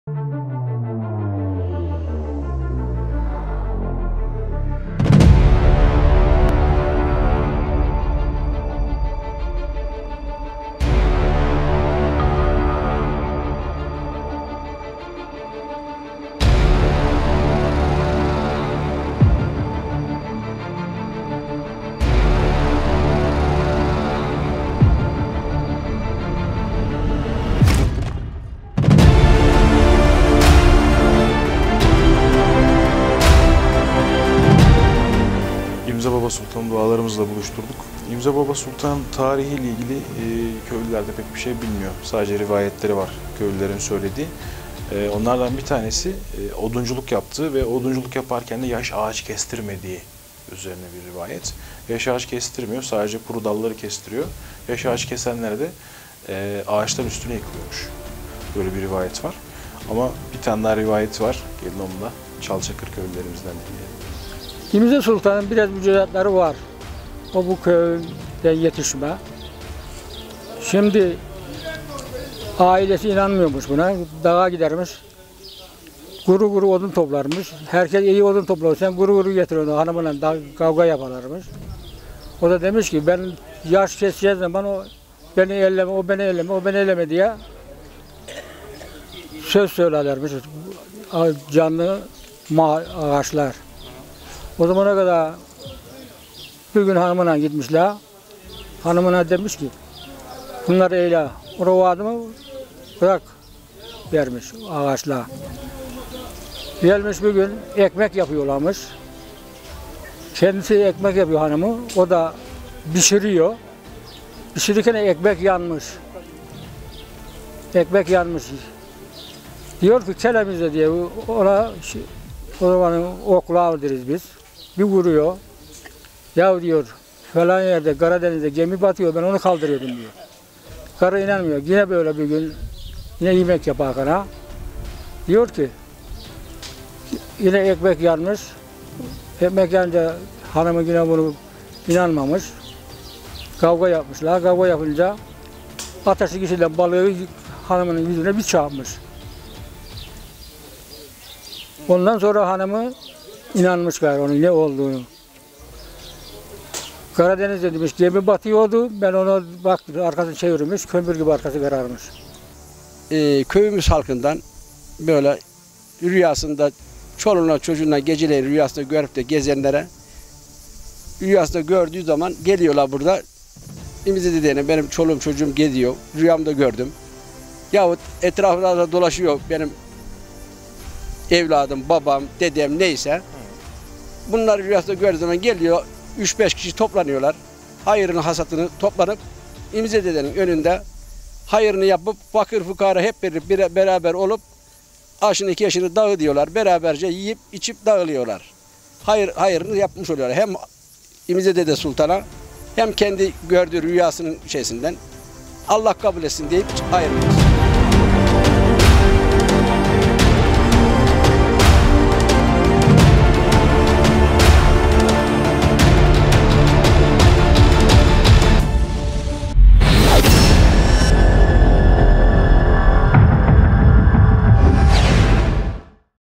İmze Dede Sultan Türbesi
Bulunduğu Yer:
Çal ilçesine bağlı Çakırlar Mahallesi mezarlığındadır. Köy meydanına geldiğinizde sol tarafa doğru giderseniz mezarlığı bulabilirsiniz.

Bina Yapısı:
Yığma taş yapılı, saç kaplamalı kubbeli, bakımlı, sergili, silidir biçimli, tek ışık
pencereli, teknik ve mimari özelliği olmayan tek odalı bir yapıdır.
Türbe içinde 2 adet yatır bulunmaktadır. bunlardan birisi imze baba sultana diğeri de karısına aittir.

Hakkındaki Bilgiler:
İmze Baba Sultan, Çakırlar Mahallesi halkındandır. Sağlığına çiftçilik ve odunculuk yaptığı, fakir bir aileye sahip olduğu ,yardım sever bir kişi olarak anlatılmaktadır. Doğum tarihi bilinmemekle beraber 1825 yılında vefat ettiği bilinmektedir.

Rivayetler:
Beyaz sakallı, yeşil kavuk ve cübbeli, uzun boylu olarak mahalle halkından bazılarının rüyasına girdiği, Dağa odun kesmeye gittiğinde yaş ağaçları kesmediği, Bir gün eşiyle beraber ekmek yaparken ekmeği yakması üzerine, eşi “Neden ekmeği yaktın” diyerek ,oklavayı vurması üzerine “Karadeniz’de bizim askerlerin gemisi batıyordu, onları kurtarmaya gittiğim için ekmek yandı” demesinden sonra, kızgınateşin içinden canlı balığı çıkartmıştır gibi rivayetler anlatılmaktadır.

Ziyaret Amaçları:
Çeşitli hastalığı olanlar tarafından şifa aramak için
Çocuğu olmayan ailelerce mutluluk aramak için
Hayır işleri yapmak için
Adanılan adakları kurban etmek için
Dileklerde bulunmak için
Yağmur duası yapmak için türbe ziyaret edilir.